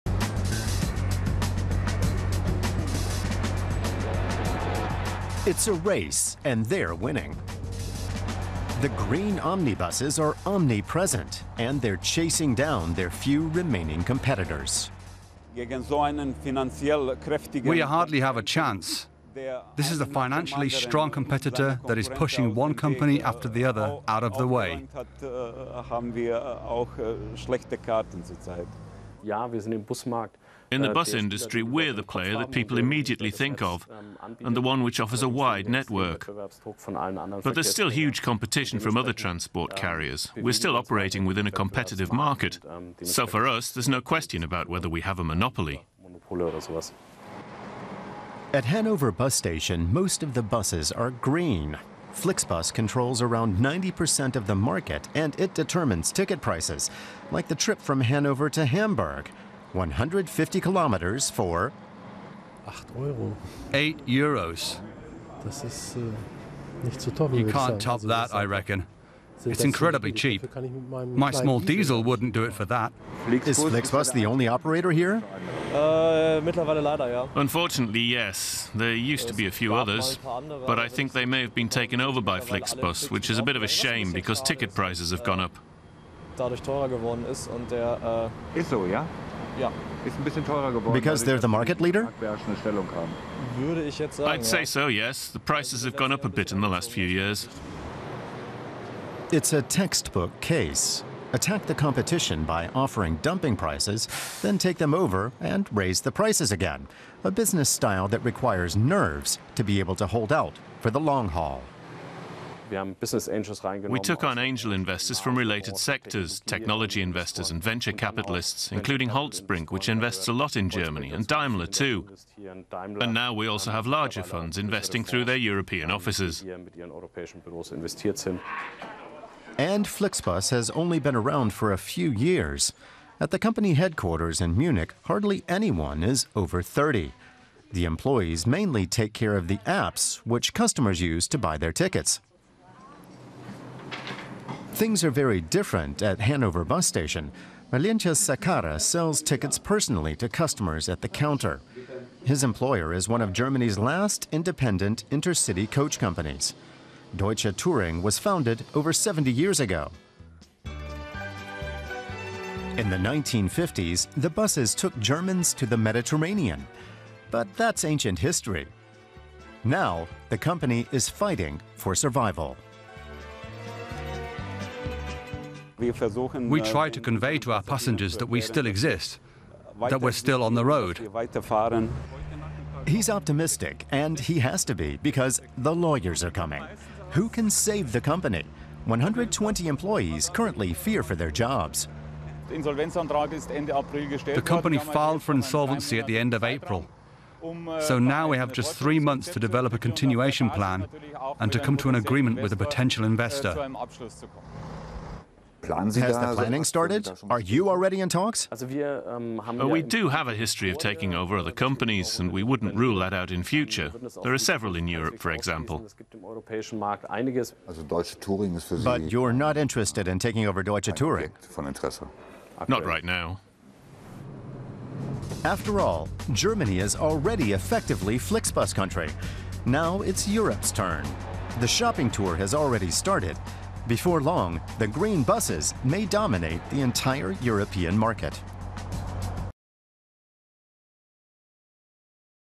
Flixbus - From start-up to market dominance | DW English
Flixbus has come to dominate the long-distance bus market in Germany, with a 90% share. It has huge pricing power and is a threat to its few remaining competitors. Now it is expanding across Europe. How has it managed to be so successful so fast?